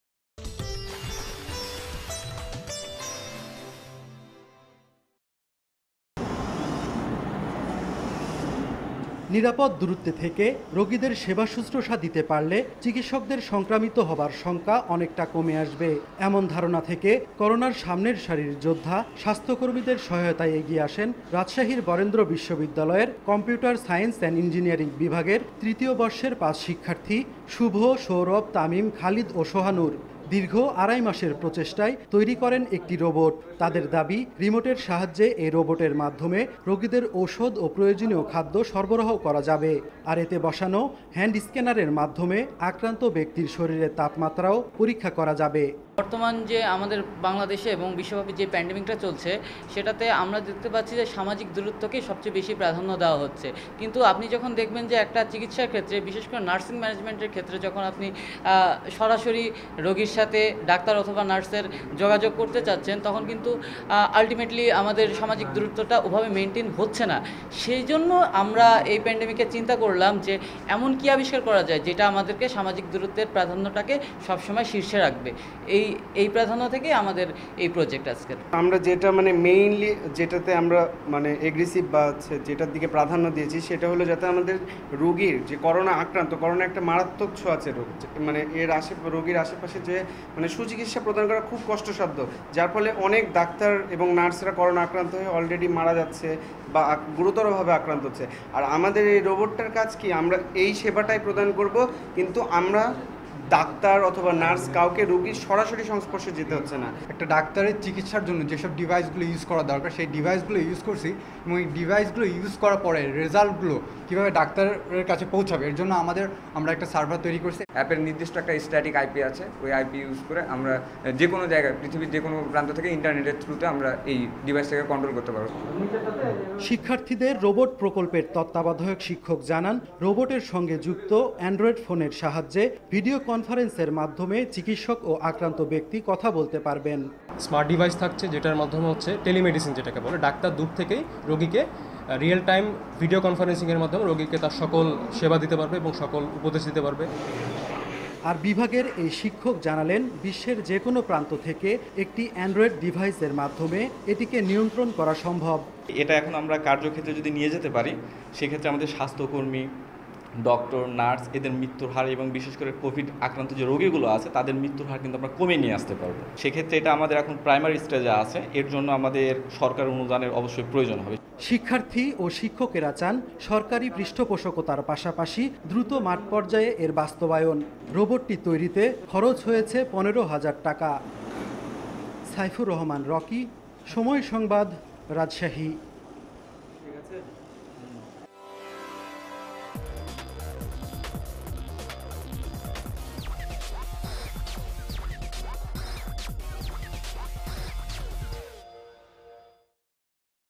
বাংলাদেশের রোবট দিবে করোনা যোদ্ধাদের সুরক্ষা ! | Bangladeshi Robot for Covid Test
Five students of the Department of Computer Science and Engineering at a private university in Rajshahi have built robots to protect Corona fighters. This robot was created after a long effort of two and a half months. It will give patients medicine and food, can also take the temperature. And doctors can sit in another room and control as well as video conferencing with the patient through a robot.